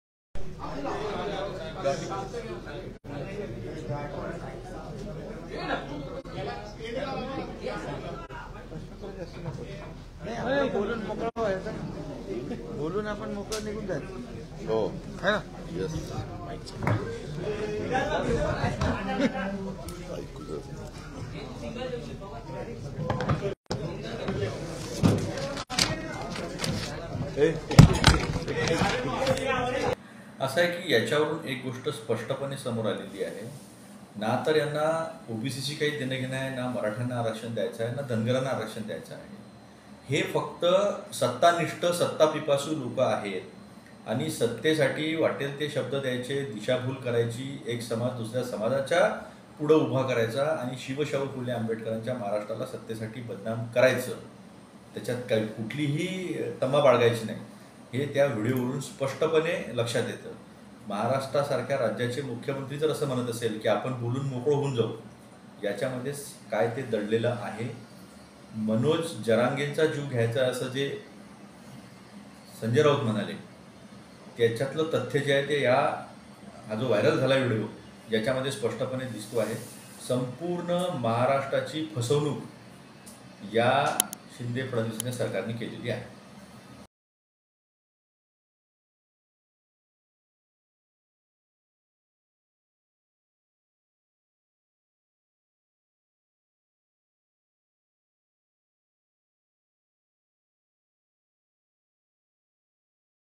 Atul Londhe on Eknath Shinde : मुख्यमंत्र्यांच्या ‘त्या’ व्हिडीओवर लोंढे भडकले | LetsUpp Marathi
आपण बोलून मोकळं व्हायचं असं वक्तव्य मुख्यमंत्री एकनाथ शिंदे यांनी मराठा आरक्षणासंबंधी बैठक झाली त्यानंतरच्या पत्रकार परिषदेच्या सुरुवातीला केलं होतं. यावरुन विरोधकांनी रान पेटवल्याचं दिसत आहे.